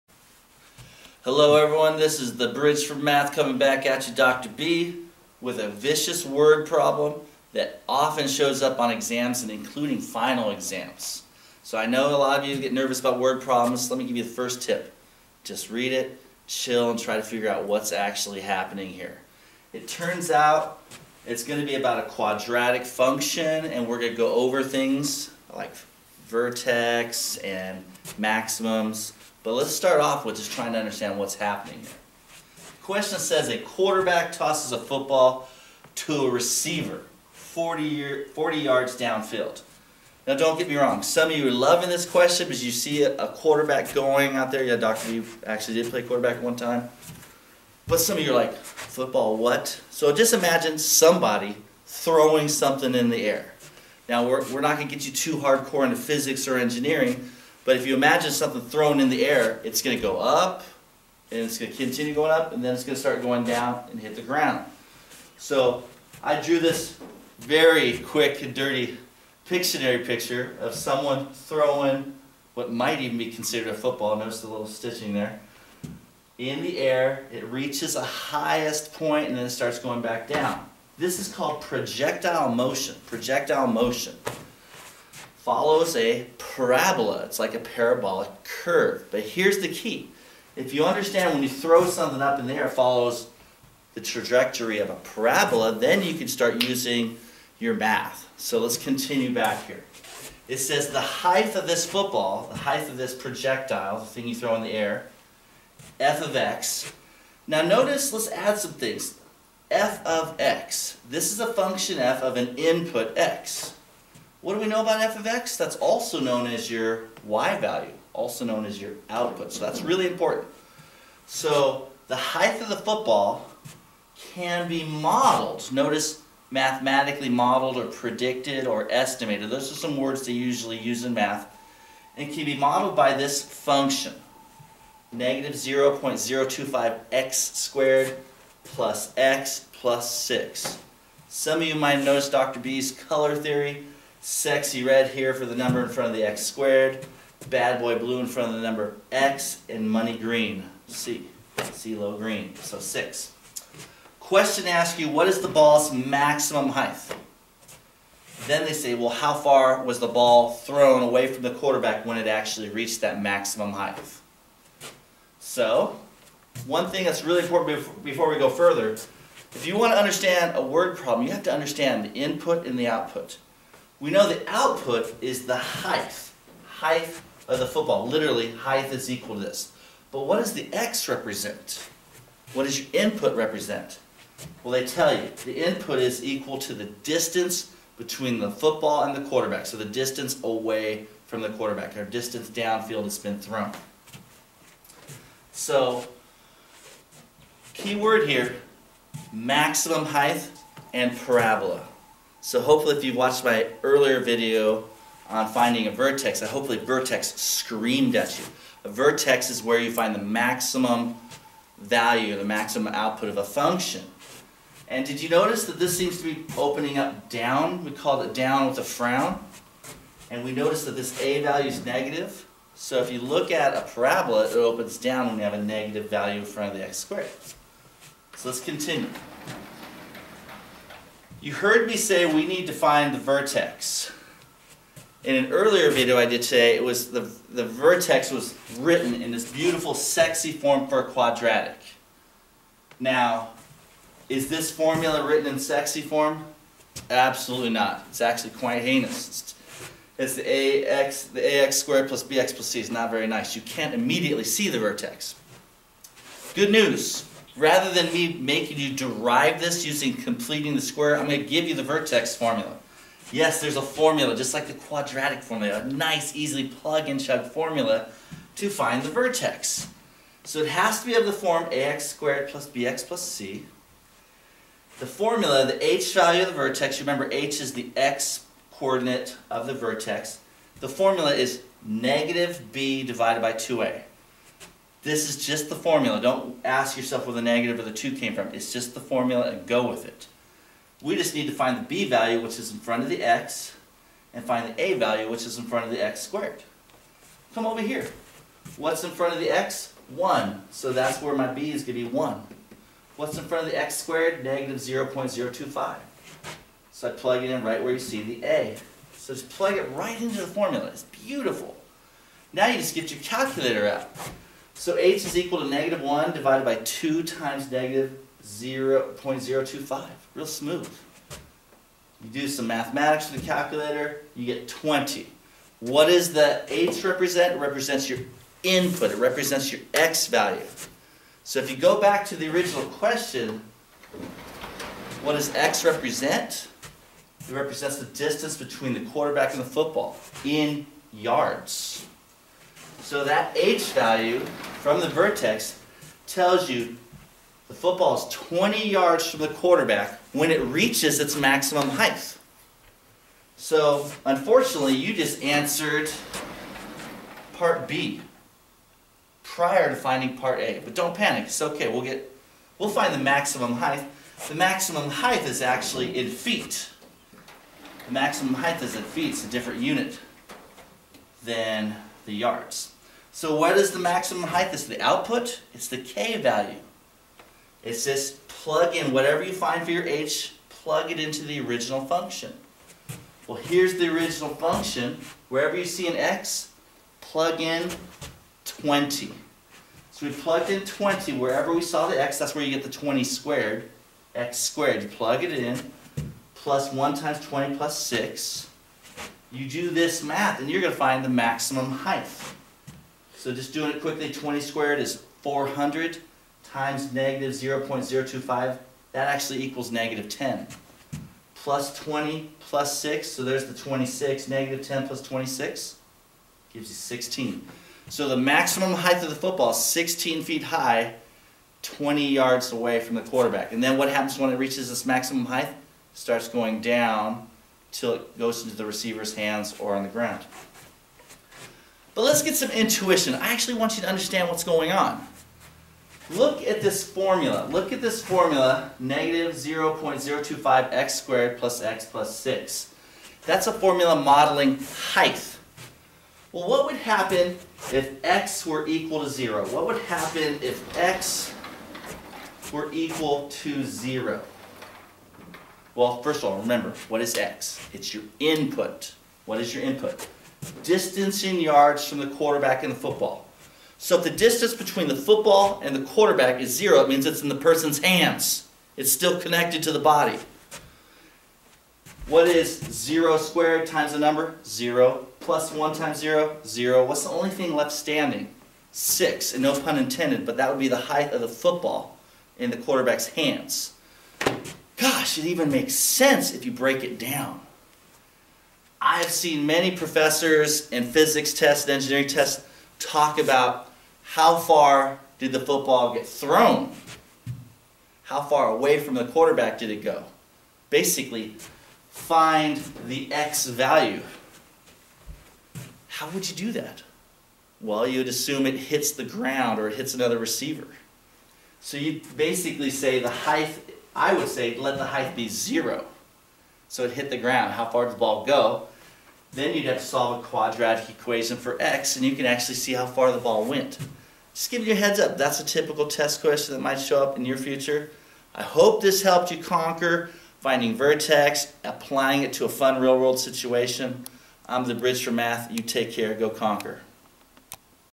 Find Maximum Value -- Word Problem, Vertex
This video tutorial goes over an interesting real-world example of finding a maximum output for a function using the vertex formula.  It is looking at the parabolic motion of a projectile (yes, the physics of a football).